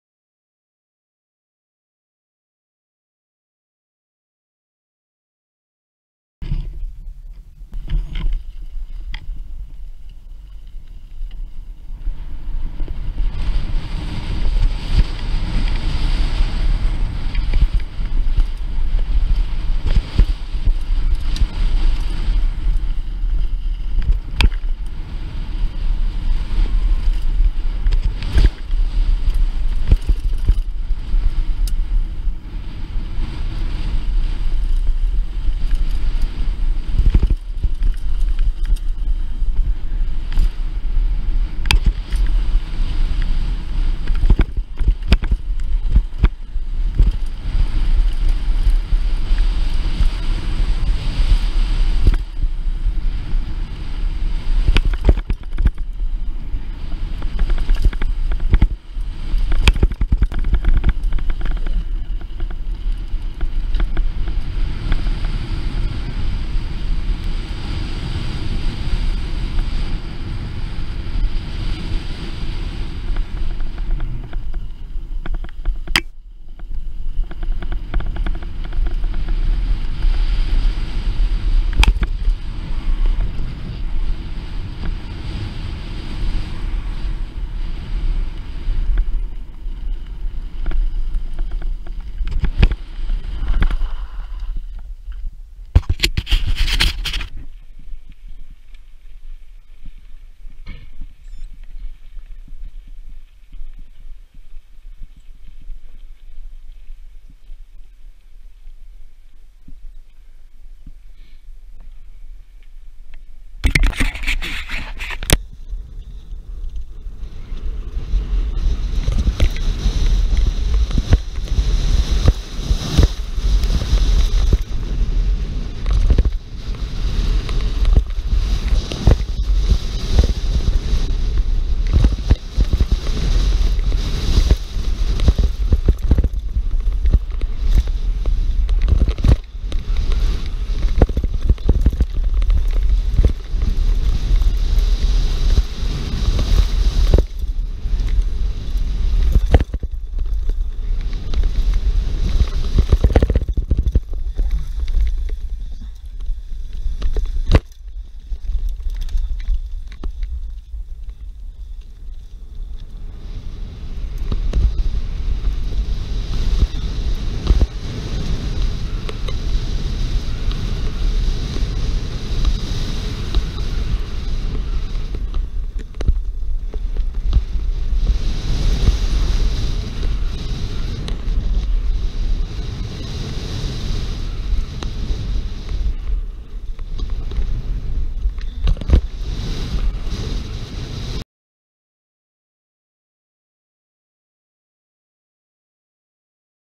Sidewinder Trail Marshall Canyon 11/25/15
Two runs down sidewinder.

The first run was slow and exploratory.  I was just getting familiar with the terrain, features, turns, and obstacles.

The second run was a little more aggressive as I was a little more familiar with the features.